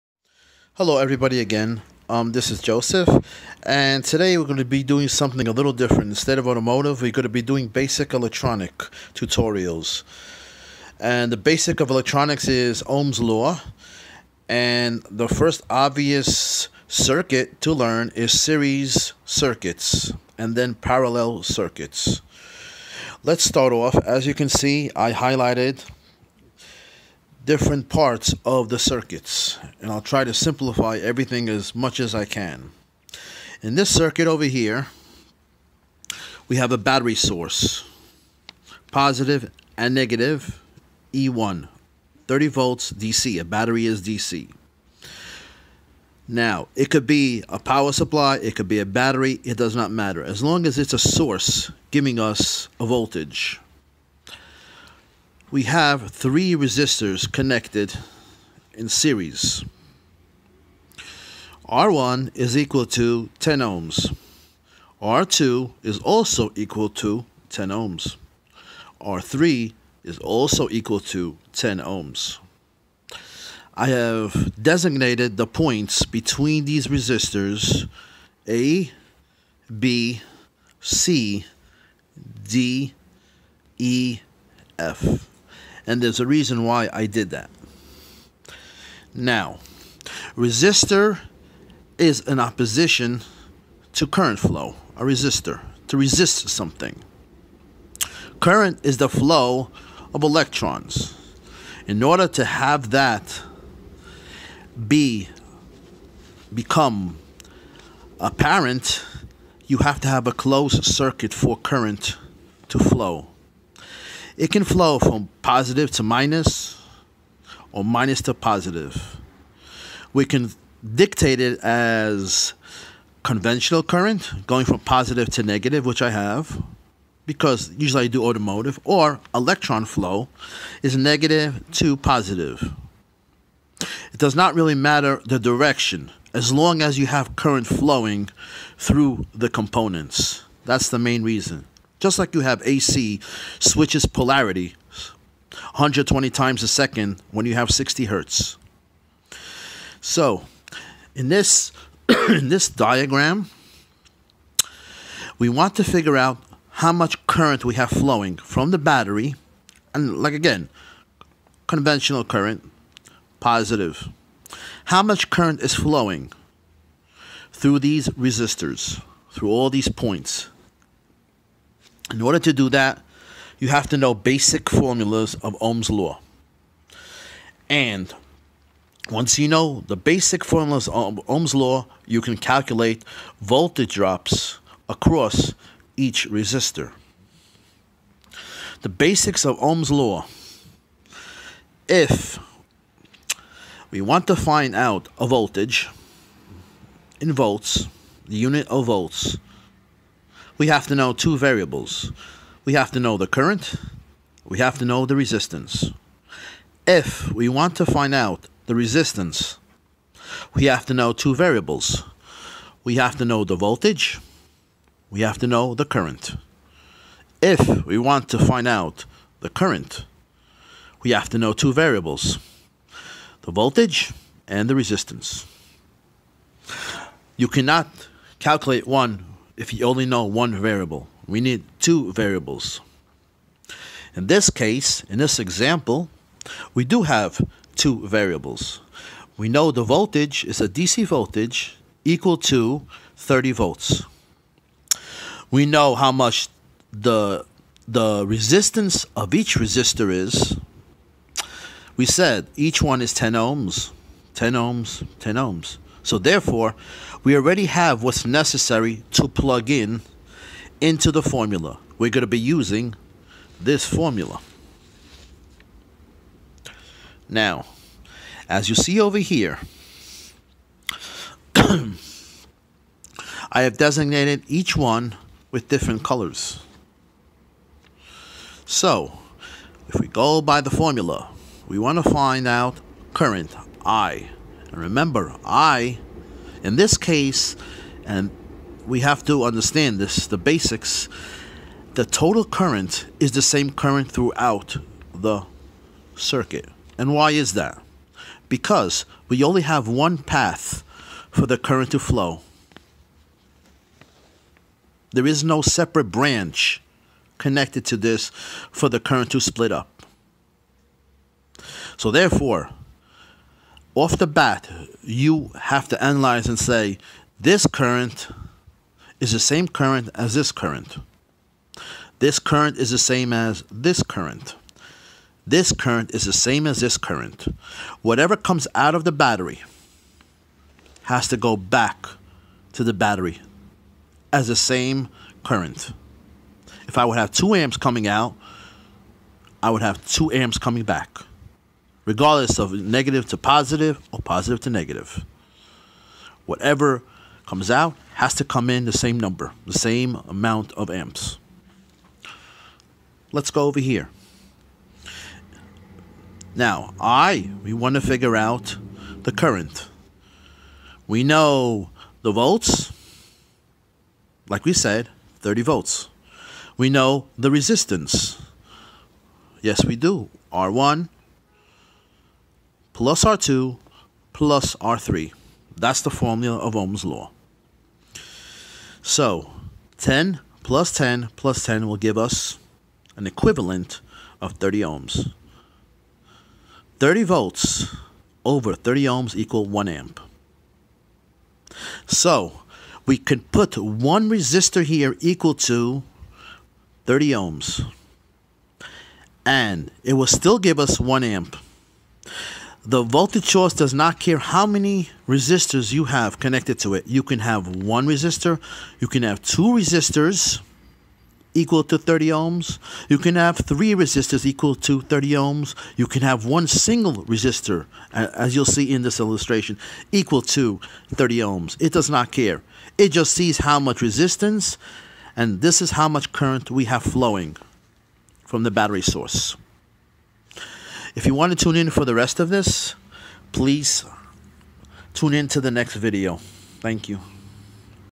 OHM'S LAW TUTORIAL SERIES CIRCUIT GOOD EXPLANATION
How ohm's law series circuits work part 1 simplify tutorial must see video. This video gives color coded wiring diagram analysis to simplify to viewe